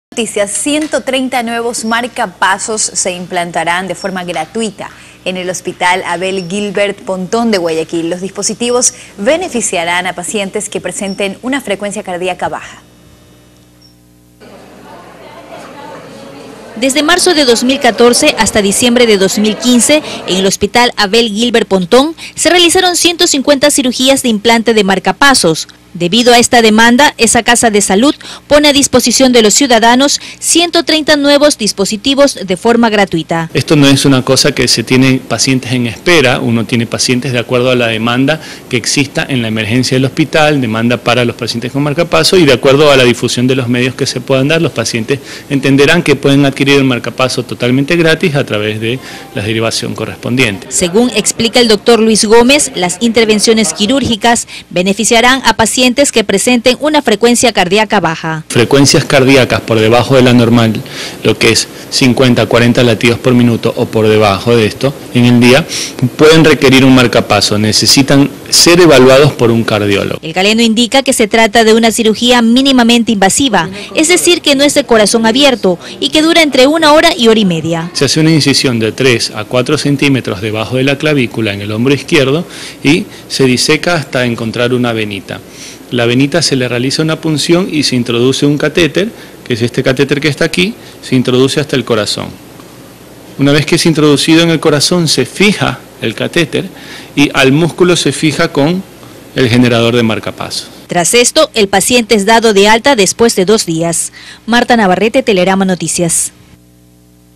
130 nuevos marcapasos se implantarán en hospital Abel Gilbert Pontón
130 nuevos marcapasos se implantaran de forma gratuita en el hospital Abel Gilbert Pontón de Guayaquil.

Los dispositivos beneficiaran a pacientes que presenten una frecuencia cardiaca baja…